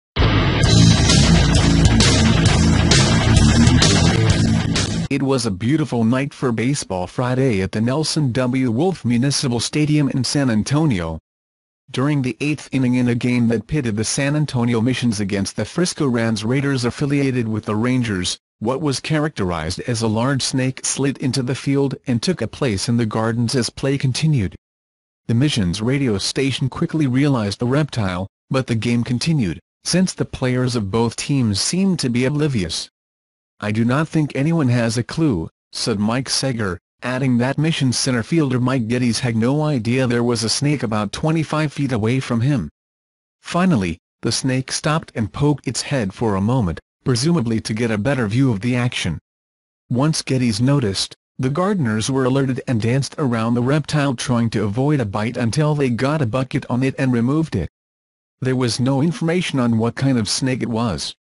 Big snake slithered on the baseball field at the Nelson W. Wolff Municipal Stadium in San Antonio.
It was a beautiful night for Baseball Friday at the Nelson W. Wolff Municipal Stadium in San Antonio. During the eighth inning in a game that pitted the San Antonio Missions against the Frisco Rans Raiders affiliated with the Rangers, what was characterized as a large snake slid into the field and took a place in the gardens as play continued. The Misiones radio station quickly realized the reptile, but the game continued, since the players of both teams seemed to be oblivious.

I do not think anyone has a clue, said Mike Saeger, adding that Missions center fielder Mike Gettys had no idea there was a snake about 25 feet away from him. Finally, the snake stopped and poked its head for a moment, presumably to get a better view of the action. Once Gettys noticed, the gardeners were alerted and danced around the reptile trying to avoid a bite until they got a bucket on it and removed it. There was no information on what kind of snake it was.